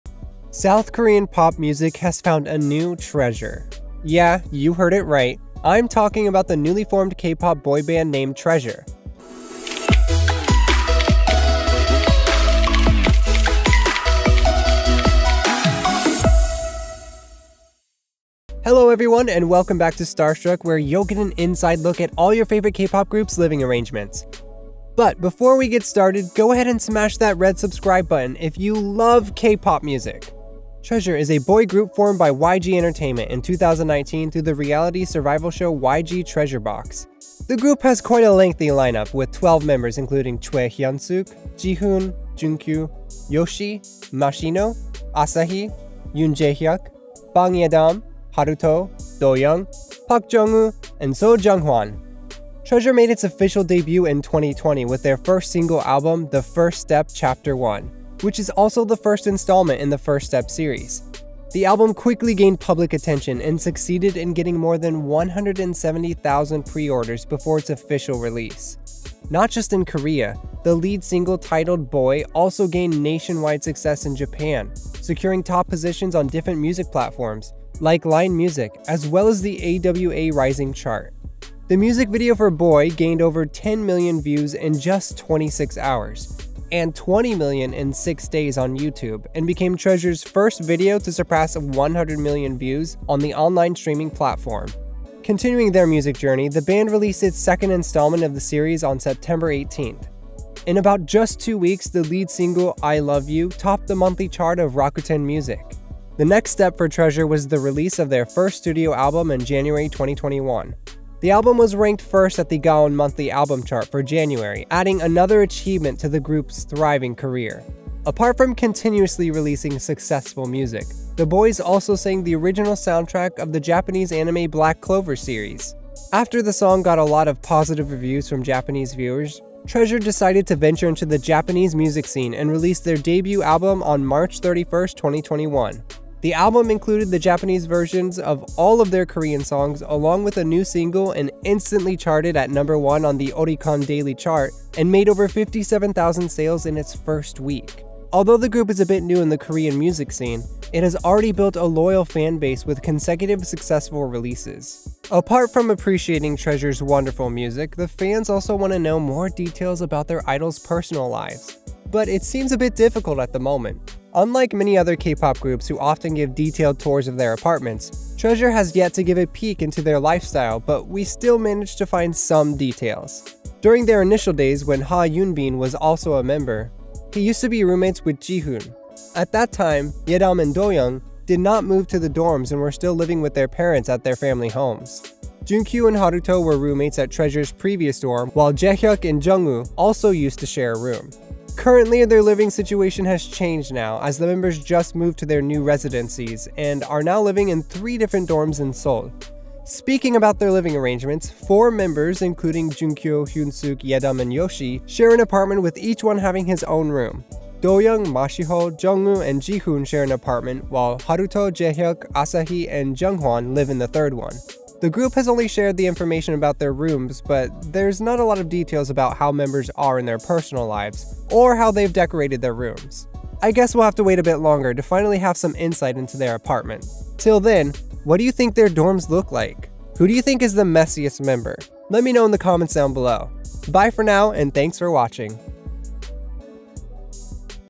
Treasure | House Tour | Their Multi-Million Dollar Dorm, Treasure's New House & More
Chart-topping K-Pop group Treasure has been blowing up the music scene. According to an outside source, these guys are super close. In this video, we will take you to see where this Kpop group calls home!